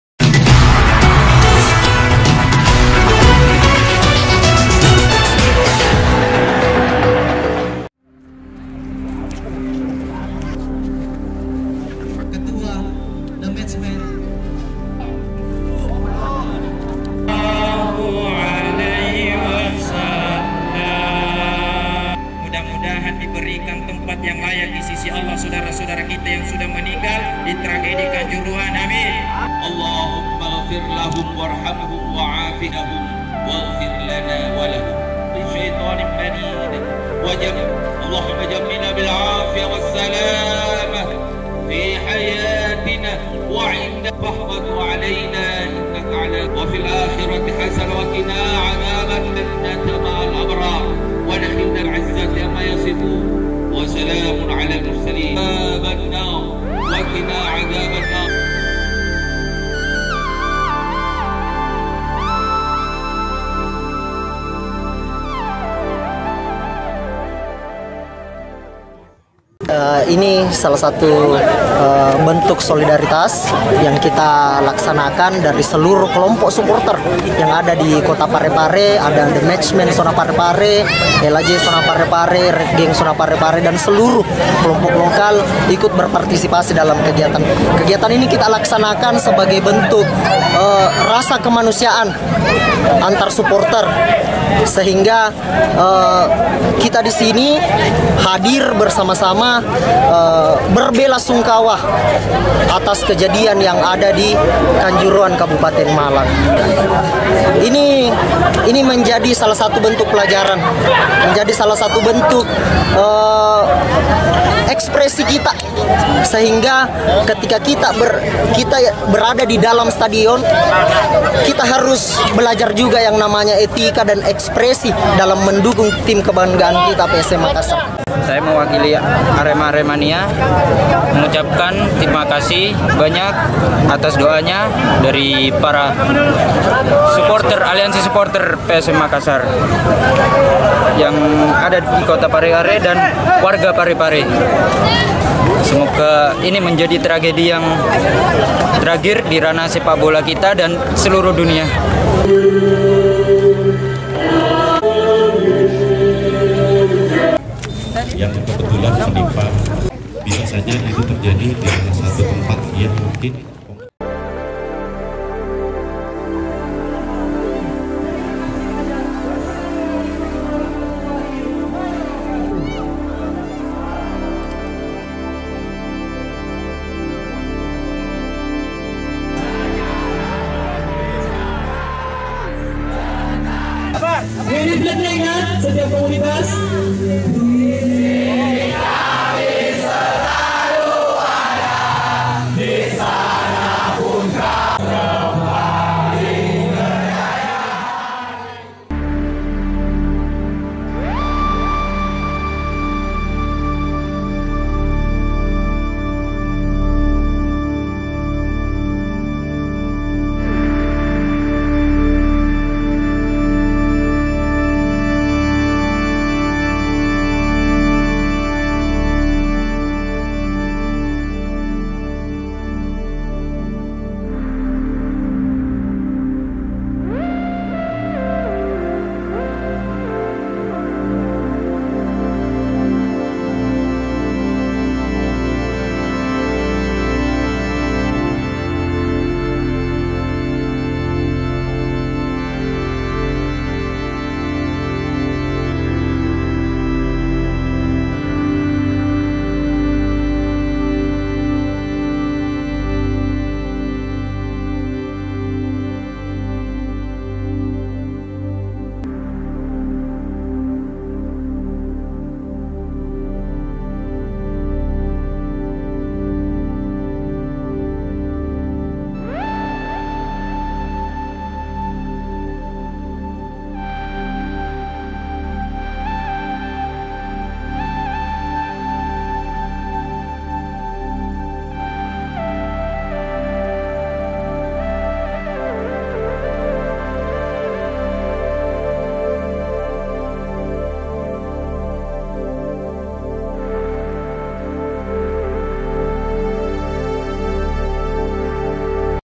TRAGEDI KANJURUHAN, ALIANSI SUPORTER PSM SE-PAREPARE GELAR DOA BERSAMA
TRAGEDI STADION KANJURUHAN MALANG JAWA TIMUR YANG MENELAN KORBAN JIWA// MASIH MENYISAHKAN DUKA MENDALAM BAGI SELURUH SUPORTER DAN KELUARGA KORBAN// UNTUK ITU ALIANSI SUPORTER PSM MAKASSAR DI KOTA PAREPARE/SULAWESI SELATAN MENGGELAR DOA BERSAMA DAN TABUR BUNGA//

Berita dan Hiburan